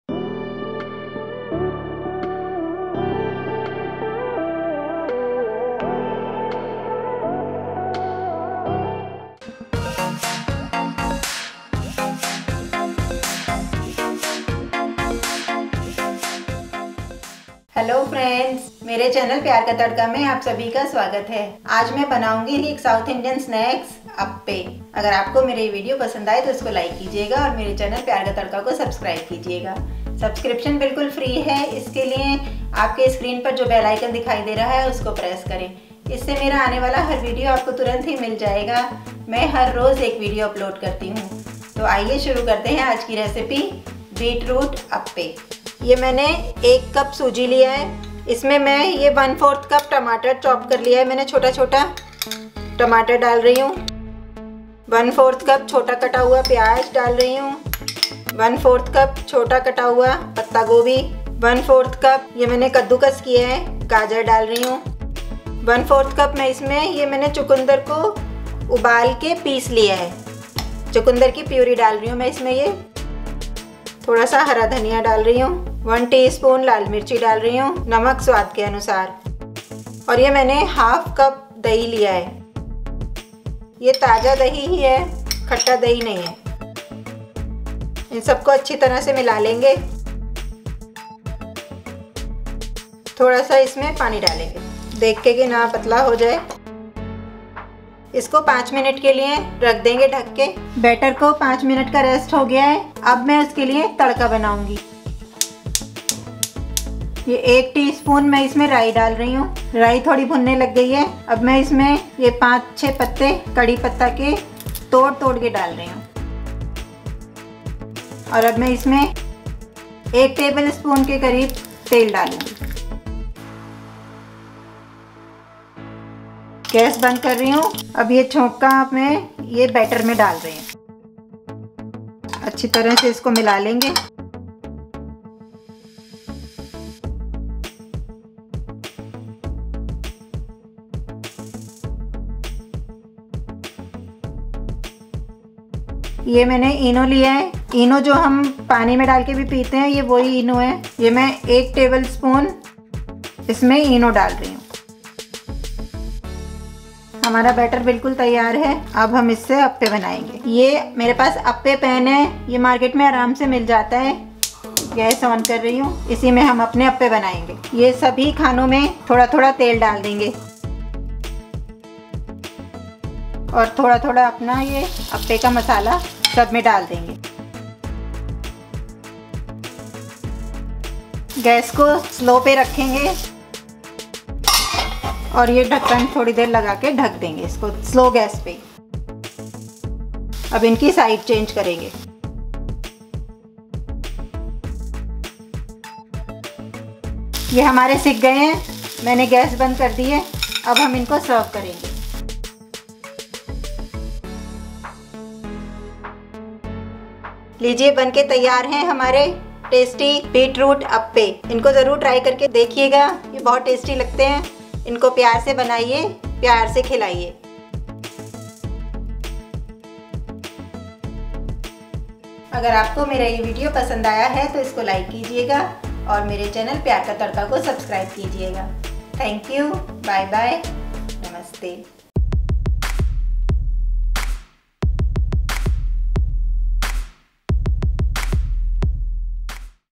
Beetroot Appe Recipe | बीटरूट अप्पे | Learn how to make Appe
In this video I bring to you Beetroot Appe. Appe is a very tasty south Indian dish.

I teach you how to add beetroot to this recipe to increase its health benefits, as well as make it tastier!